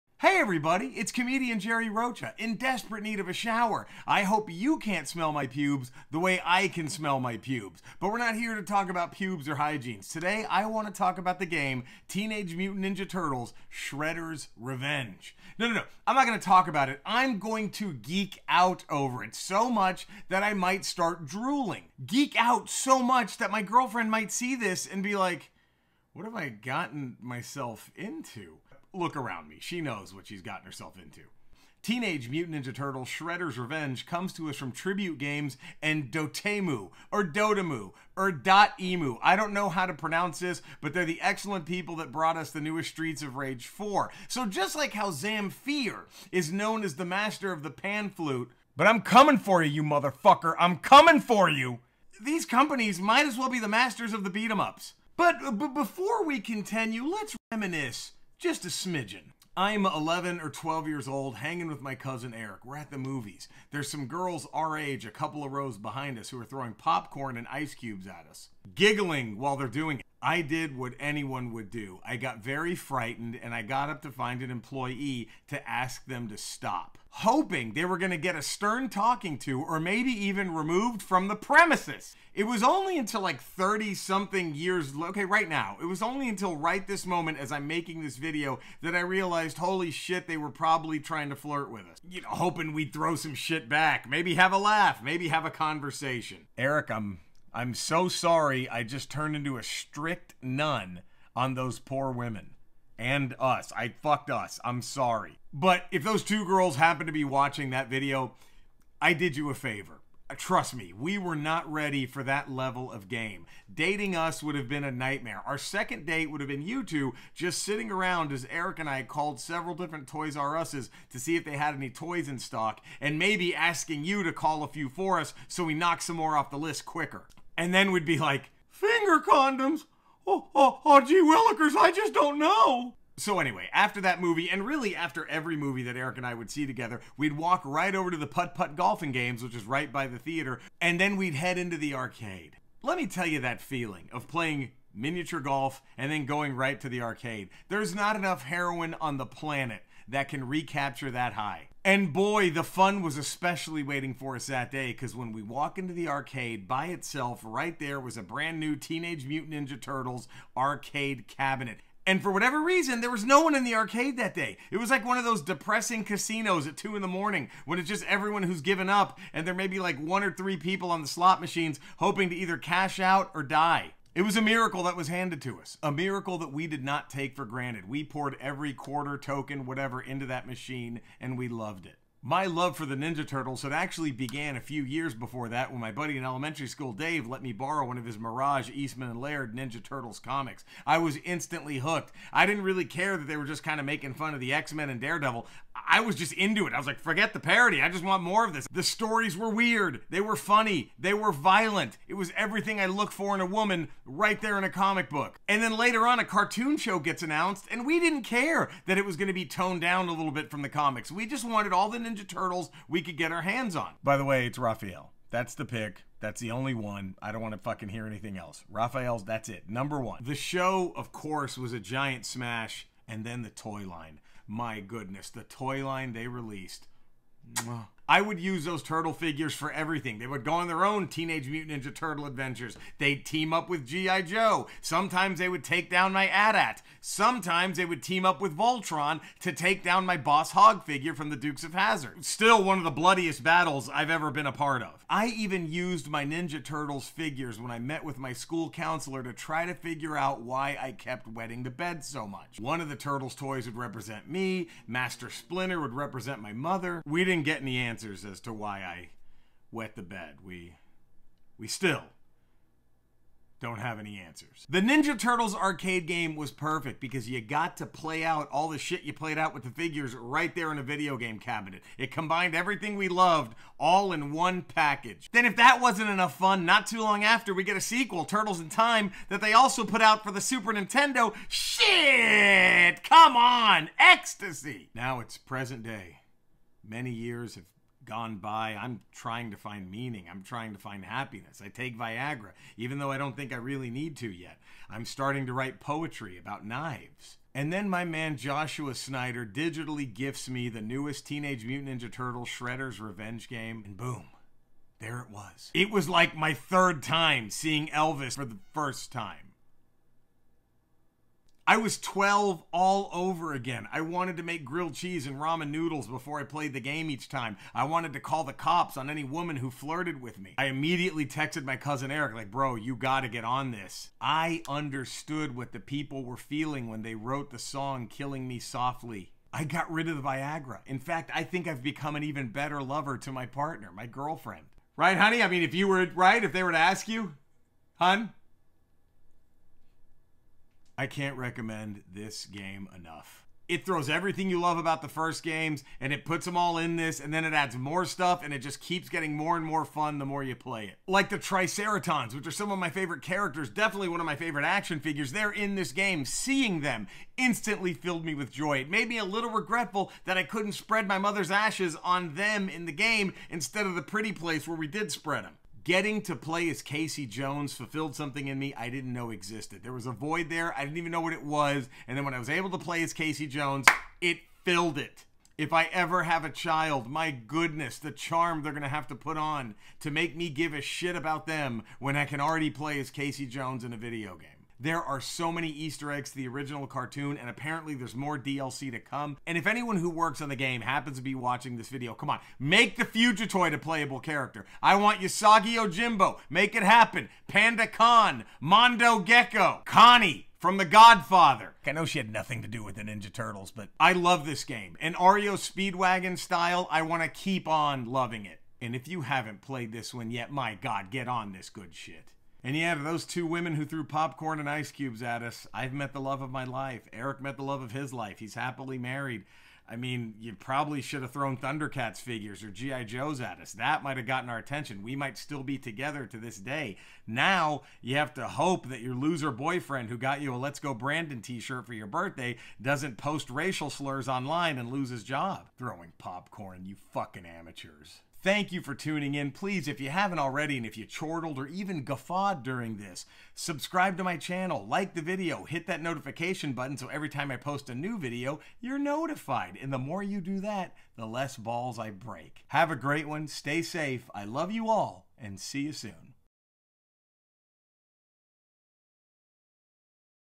TMNT: Shredder's Revenge
In this one, I talk about how much I love the amazing TMNT game shreddersrevenge enjoy!